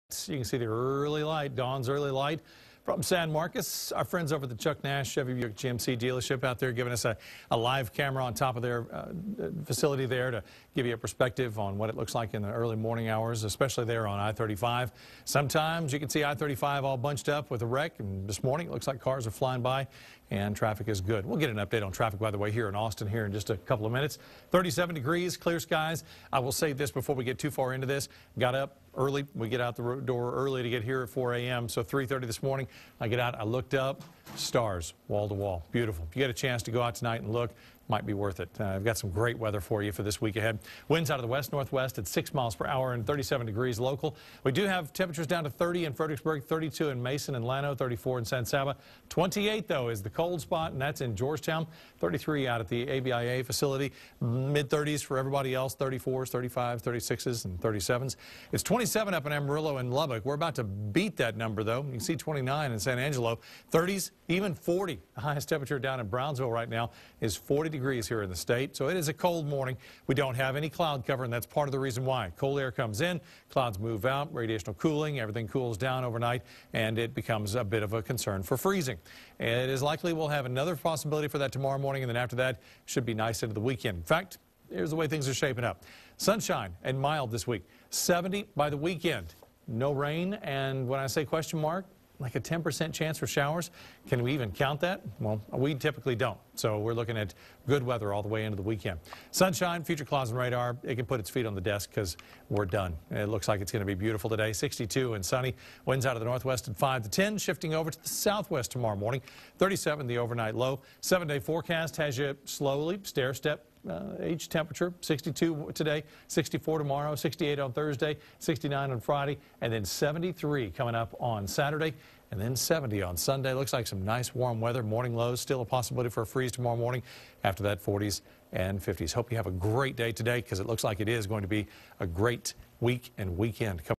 Tuesday Morning Forecast
Austin News Today's Tuesday morning weather forecast with Shawn Rutherford.